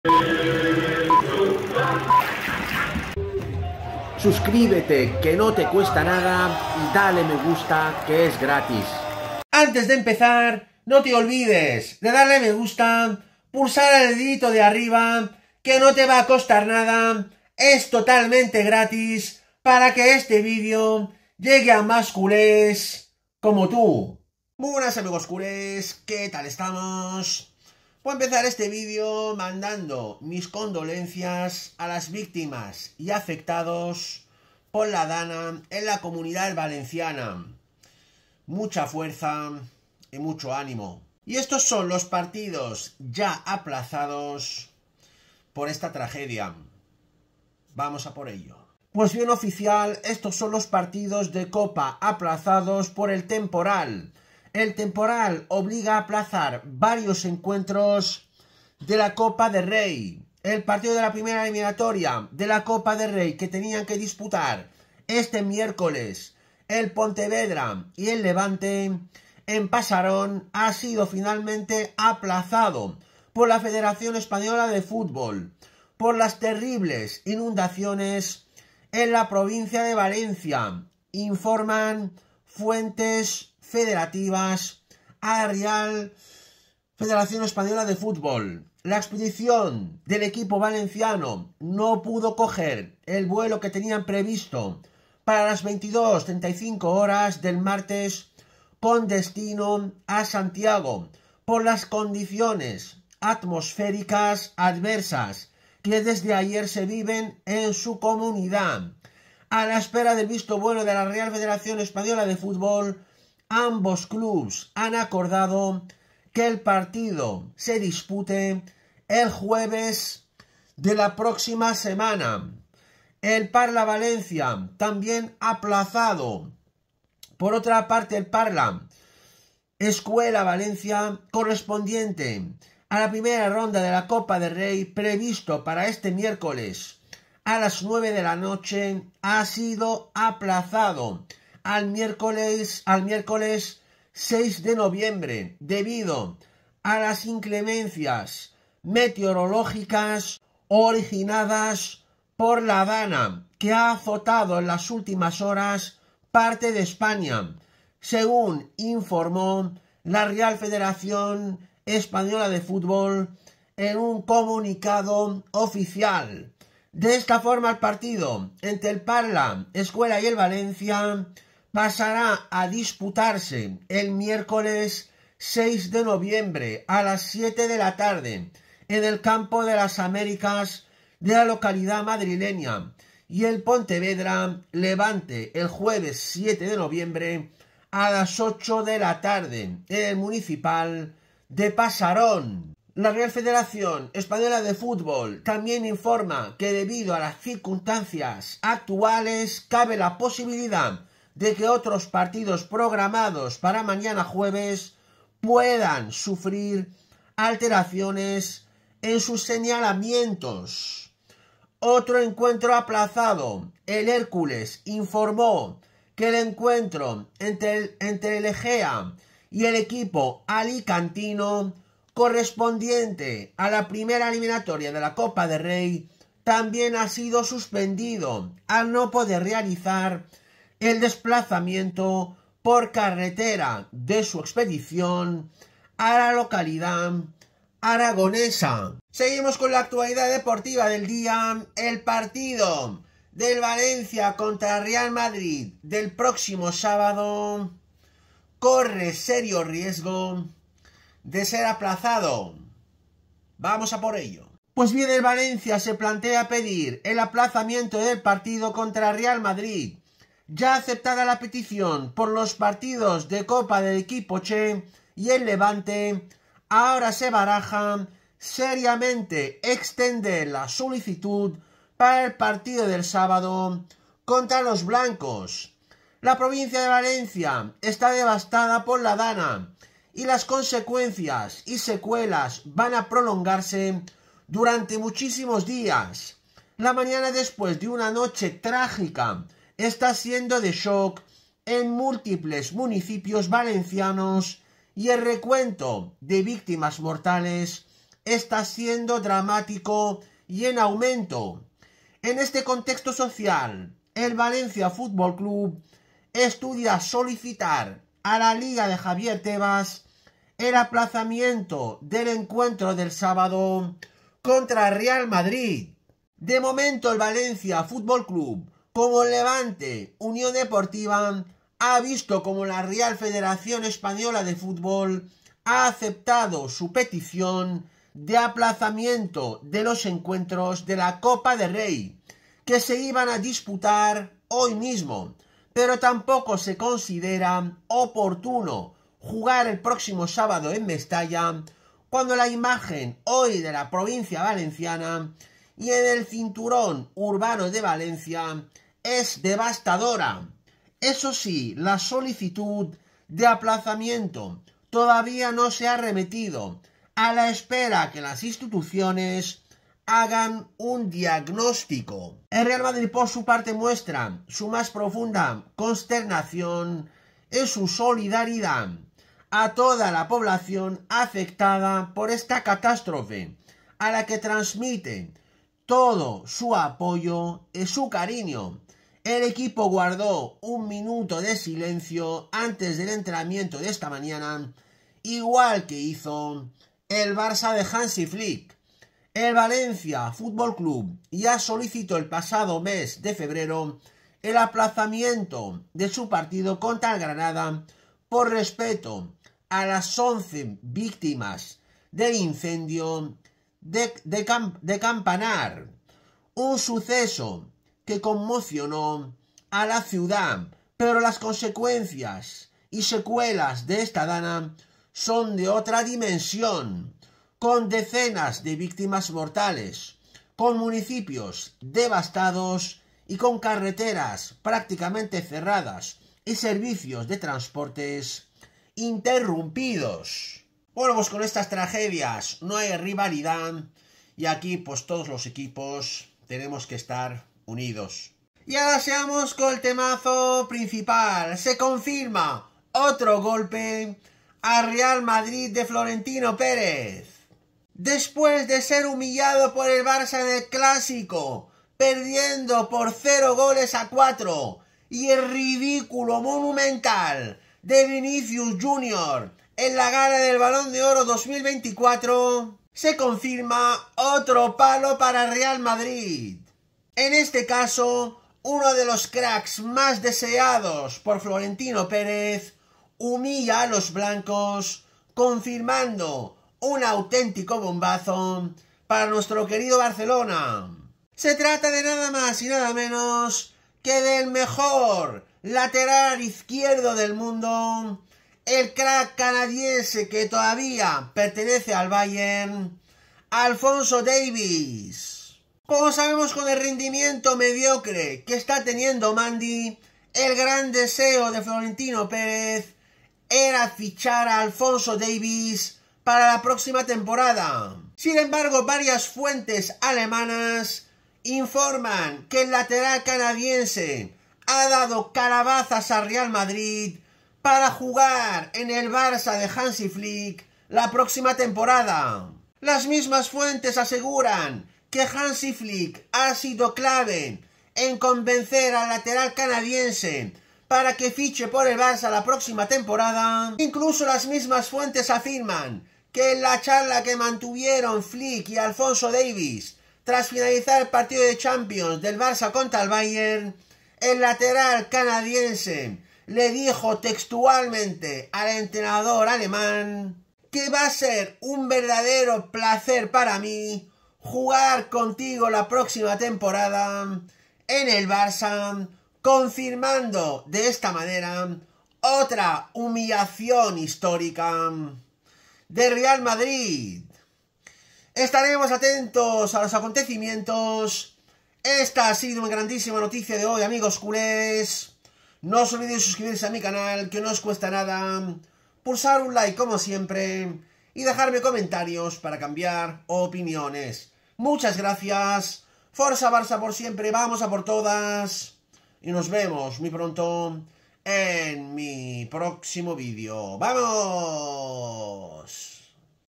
💥😱 BOMBAZO! EL CRACK MÁS DESEADO DE FLORENTINO HUMILLA AL MADRID CONFIRMANDO SU FICHAJE POR EL BARÇA
Bombazo en el Barcelona, sorpresa mundial. El Crack más deseado de Florentino humilla al Real Madrid confirmando su fichaje por el Barça paralizando el Fútbol mundial